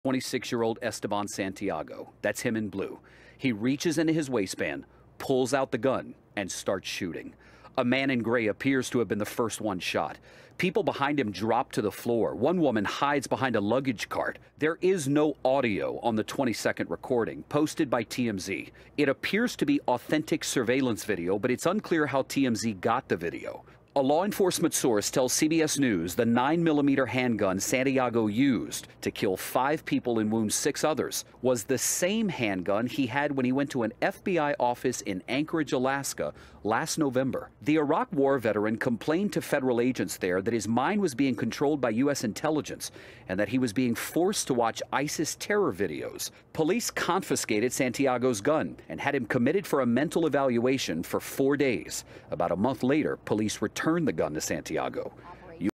VIDEO shows moment Fla gunman opened fire..
Newly released surveillance video shows the moment a gunman opened fire at the baggage claim area of Fort Lauderdale-Hollywood International Airport on Friday, killing five people and wounding six others.
In the short video, a man in a blue shirt can be seen walking with other people in the baggage claim of the airport before suddenly reaching into his waistband, pulling out a handgun, and starting to fire.
Travelers can be seen at first standing still in reaction to the shots, before then proceeding to take cover. Some people dive behind luggage carts and baggage carousels, while others run for their lives.